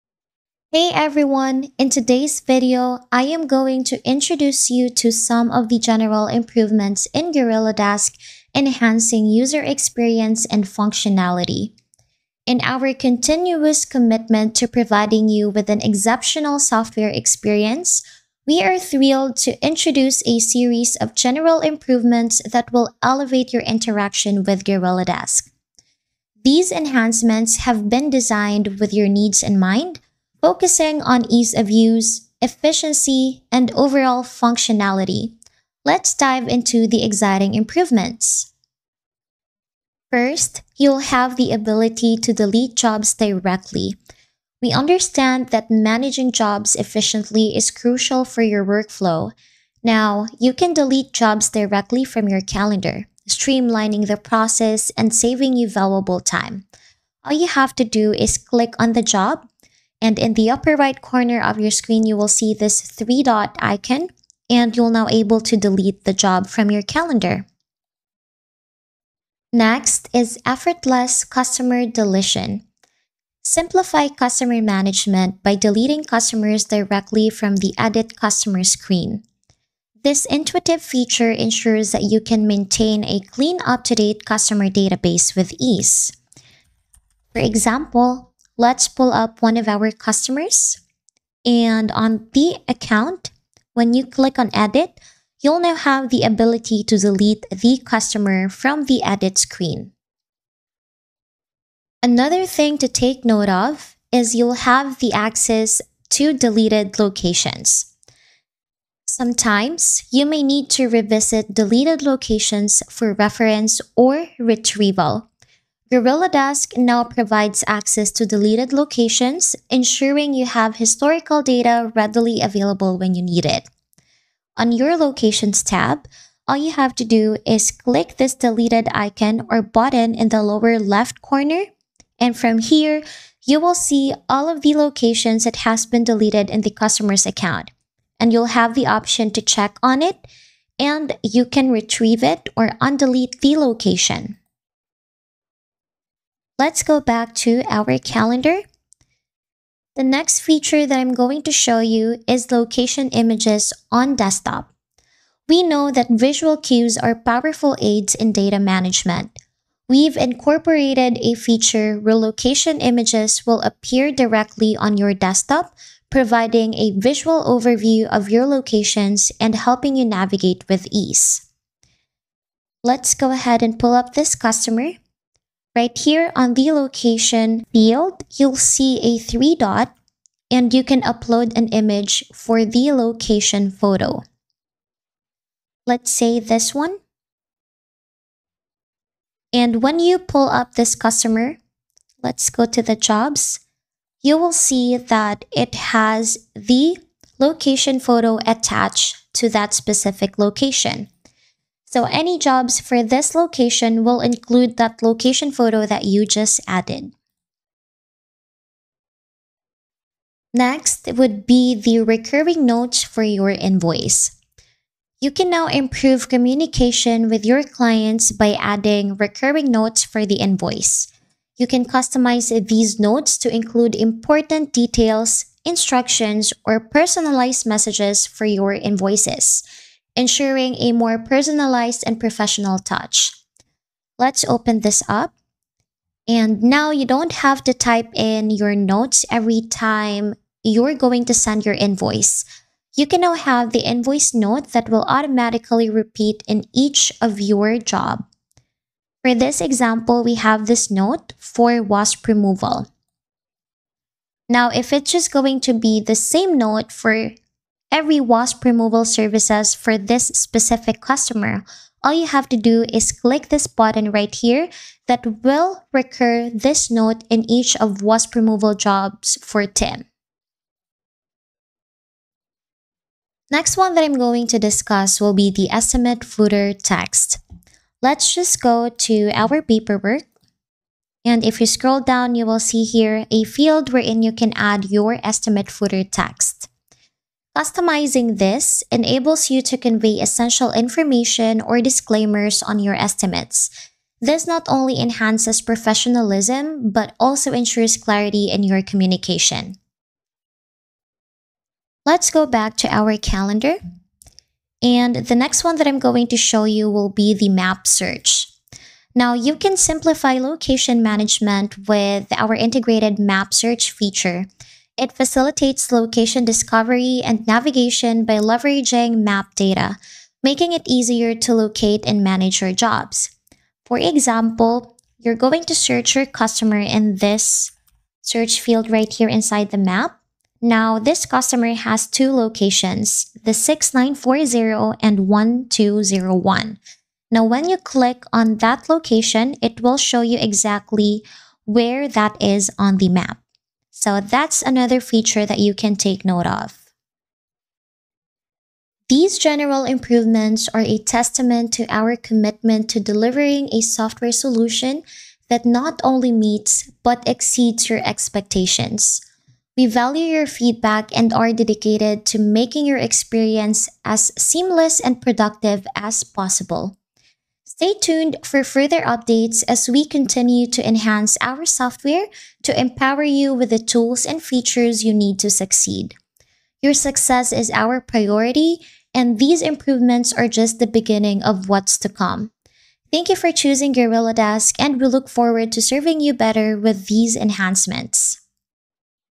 General Improvements in GorillaDesk
0:00 - Intro
0:40 - Delete Jobs Directly from Calendar
01:09 - Effortless Customer Deletion
01:41- Access to Deleted Locations
02:29 - Location Images on Desktop
03:30 - Recurring Invoice Notes
04:33 - Estimate Footer Text
05:04 - Map Search
05:49 - Give us a Call